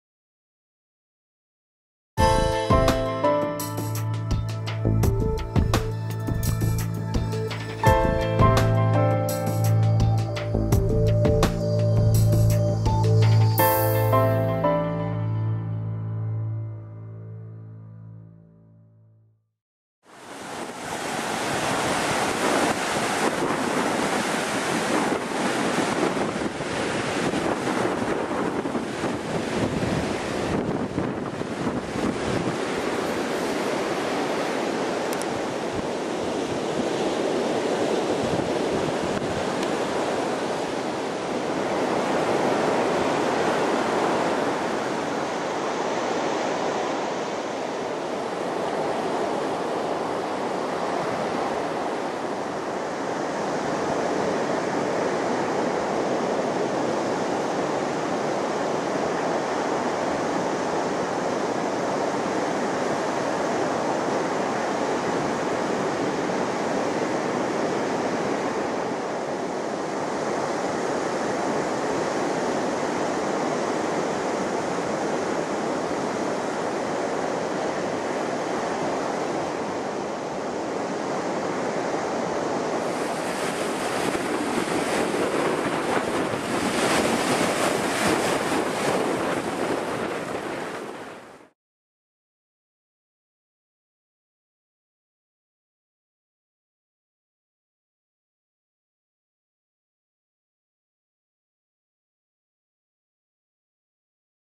Estrella de Mar (Isla de El Hierro)
Avistamiento de una Estrella de Mar Común (Marthasterias glacialis) en la Playa de Arenas Blancas de la isla de El Hierro. Grabación realizada por Miguel Ángel López Varona el 30 de mayo de 2013 para el Blog "Cuaderno de Campo del Treparricos", durante la travesía por la isla de El Hierro. Más información en: htpp://